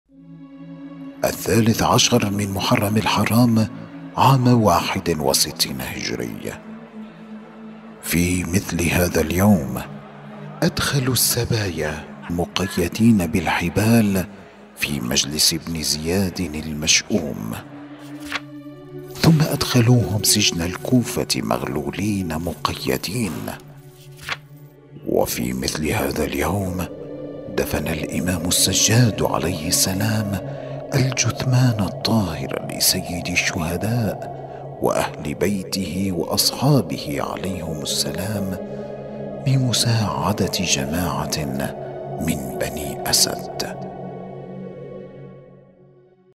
في مثل هذا اليوم - الثالث عشر من محرم الحرام عام 61 للهجرة
🏴  13 محرم الحرام
في مثل هذا اليوم أدخلوا السبايا مقيّدين بالحبال في مجلس ابن زياد المشؤوم، ثمّ أدخلوهم سجن الكوفة مغلولين مقيّدين.
وفي مثل هذا اليوم دفن الإمام السجاد عليه السلام الجثمان الطاهر لسيد الشهداء وأهل بيته وأصحابه عليهم السلام بمساعدة جماعة من بني أسد.